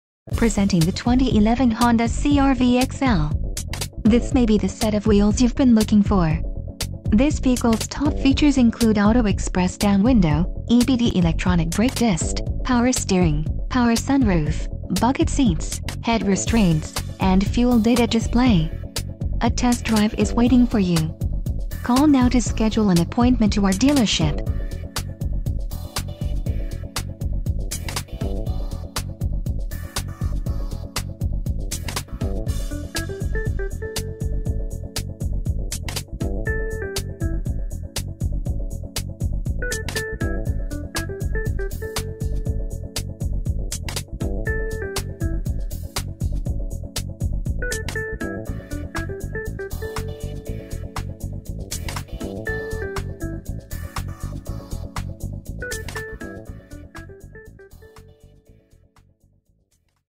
2011 Honda CR-V - Burns Honda - Marlton, NJ 08053
Presenting the 2011 Honda CR-V EX-L. This may be the set of wheels you've been looking for. This vehicles top features include Auto Express Down Window, EBD Electronic Brake Dist, Power Steering, Power Sunroof, Bucket Seats, Head Restraints, and Fuel Data Display. A test drive is waiting for you.